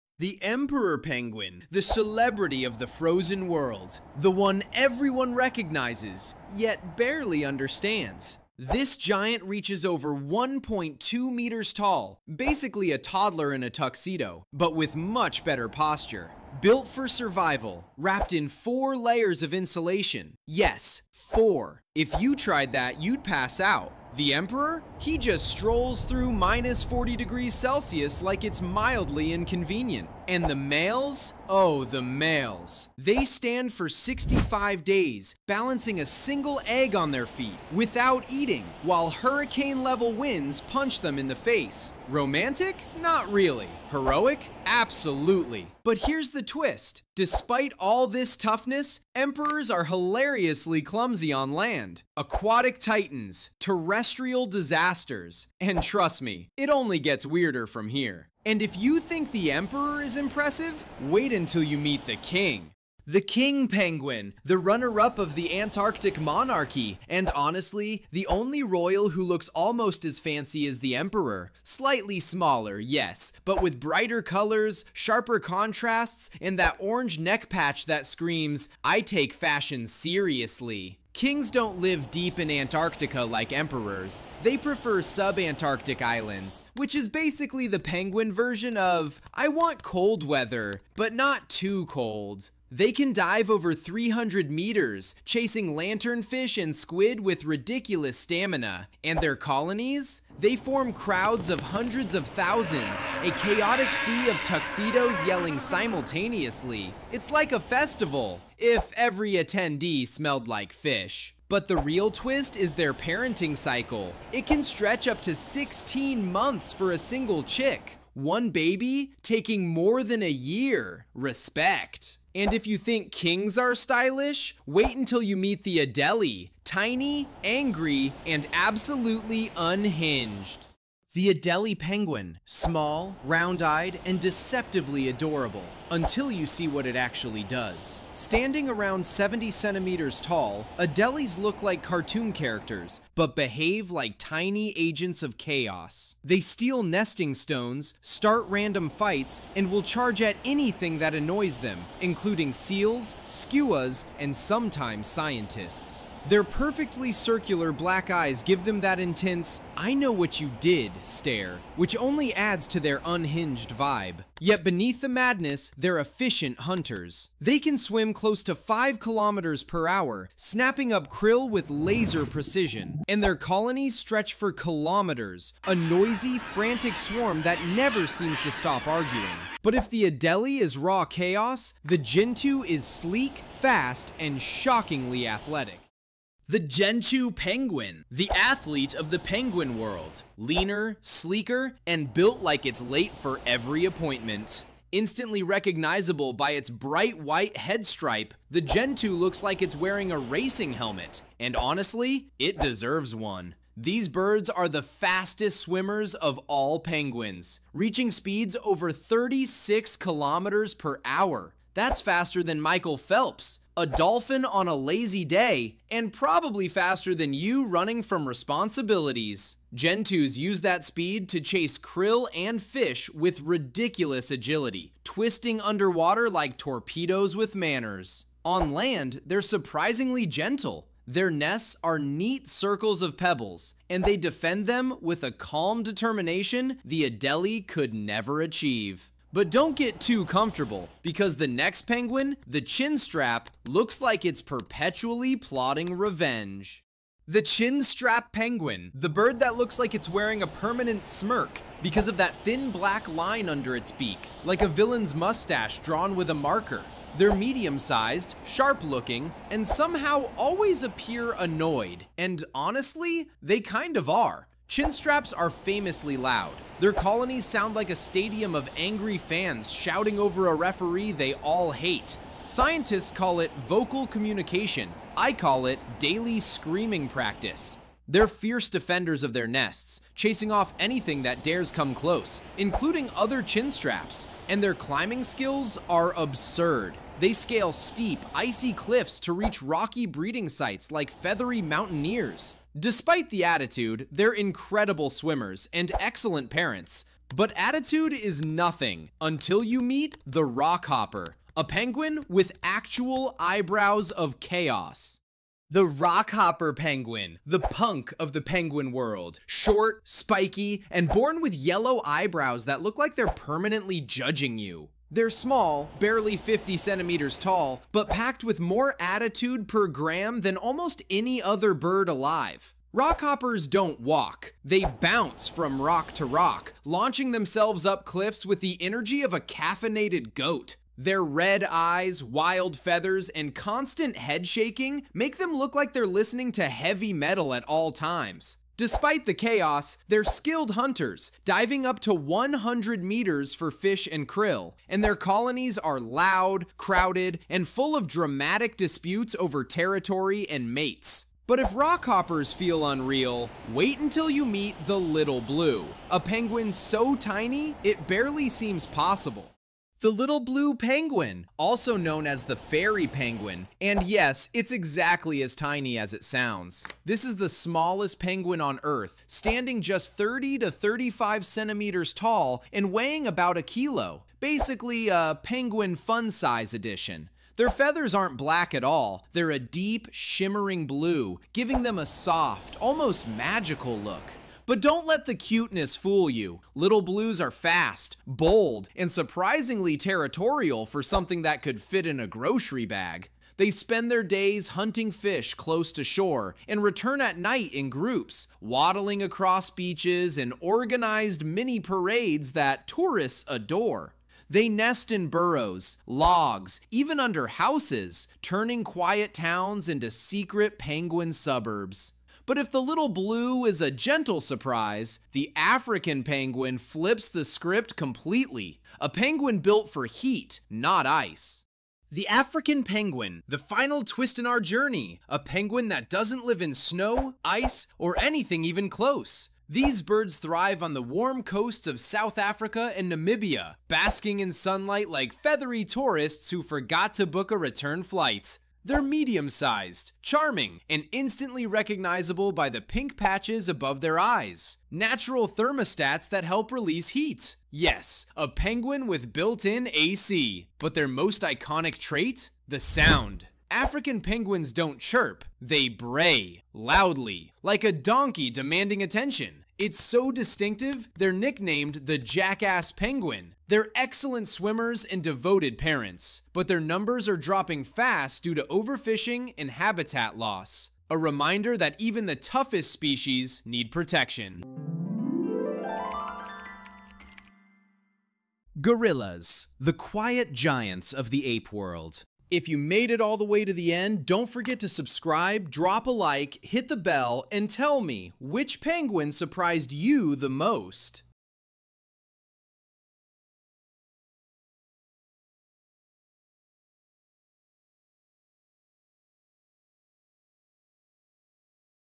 Every Penguin Species Explained in 8 Minutes
🌍 Every Penguin Species Explained in 8 Minutes — from Emperor to Little Blue.

📚 Dive into the icy, chaotic, and surprisingly diverse world of penguins in Every Penguin Explained in 08 Minutes. From the towering Emperor Penguin to the tiny Little Blue, this video breaks down all major species with fast-paced explanations, fun facts, and stunning visuals. If you’ve ever wondered what makes each penguin unique — size, behavior, habitat, or personality — this is your ultimate crash course.

🐧 You’ll learn about their wild adaptations, their unbelievable survival strategies, and the hilarious quirks that documentaries never fully explain. Whether you’re searching for “types of penguins,” “how many penguin species exist,” or simply looking for a quick and complete guide to penguins, this video delivers everything in a clear and engaging way. Perfect for wildlife fans, students, nature lovers, and anyone curious about these iconic birds.

🎬 Get ready to dive into icy worlds and discover the wild lives of every penguin species ❄️.

Chapters:
00:00 - Emperor Penguin
01:03 - King Penguin
02:11 - Adelie Penguin
03:07 - Gentoo Penguin
04:07 - Chinstrap Penguin
05:06 - Rockhopper Penguin
06:00 - Little Blue Penguin
07:02 - African Penguin
07:54 - Next Episode
07:58 - Please Subscribe

💭 Which penguin surprised you the most?

👉 Watch until the end and discover every hidden detail.